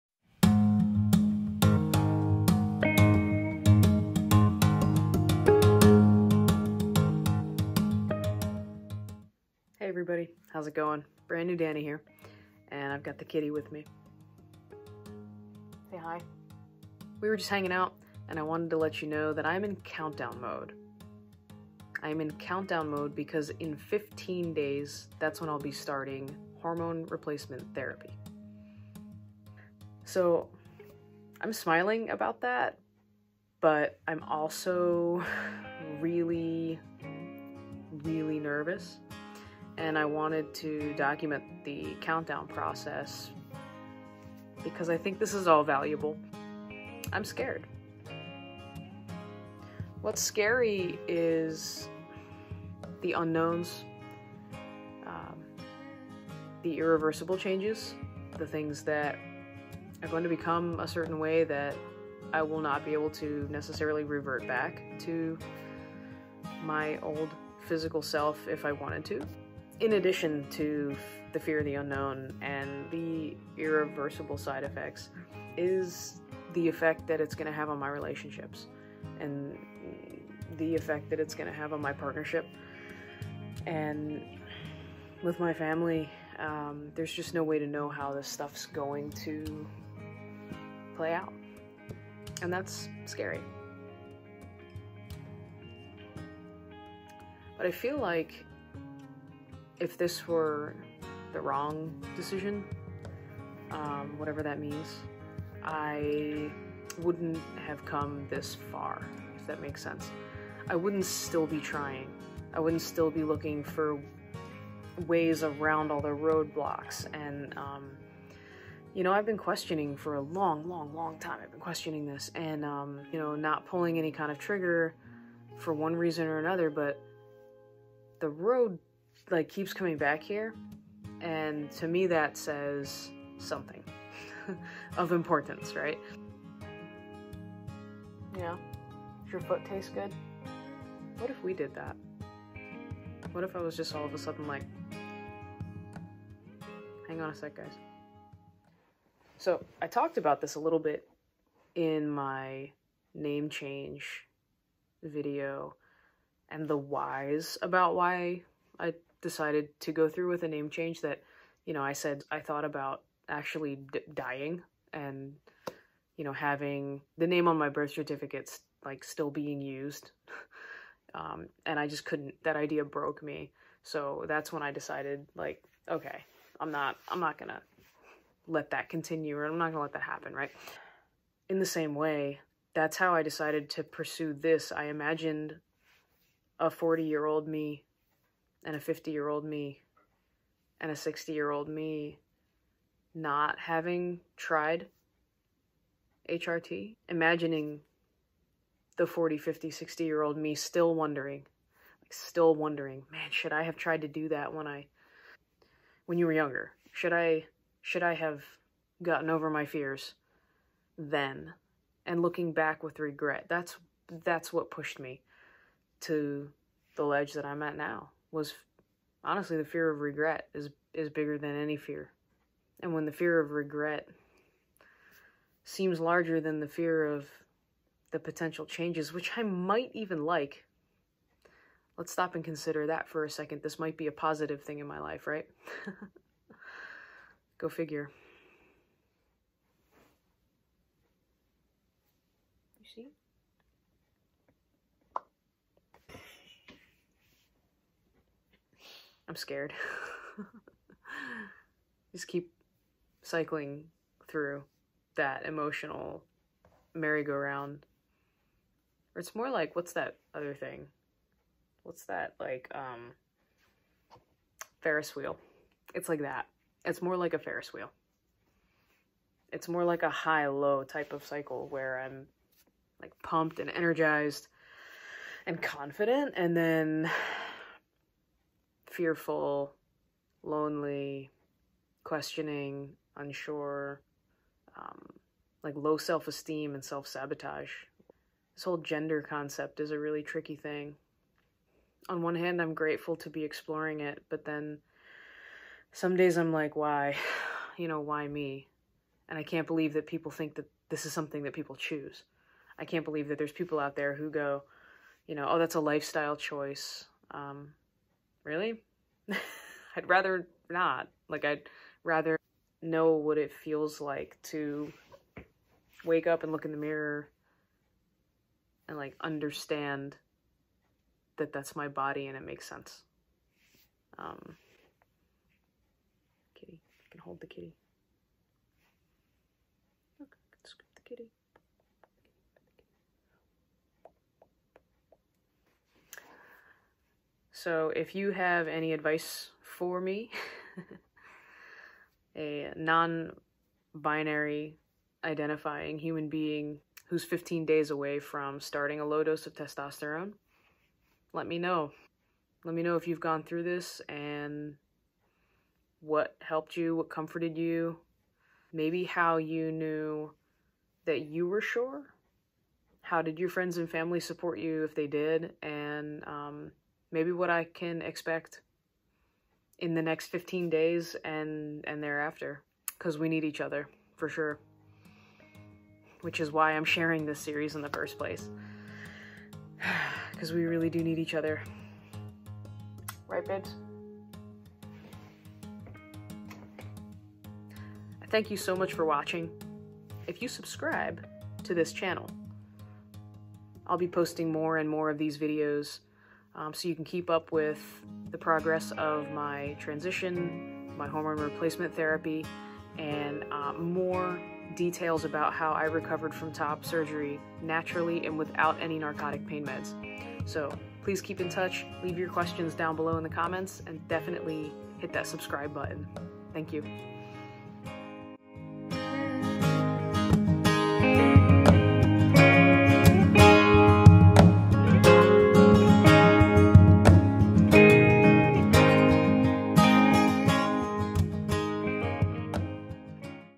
Countdown to First Testosterone Shot | Non-Binary Transition | HRT | Brand New Dani Ep. 4
In this Vlog I discuss my feelings in the 15 days prior to starting testosterone HRT for the first time.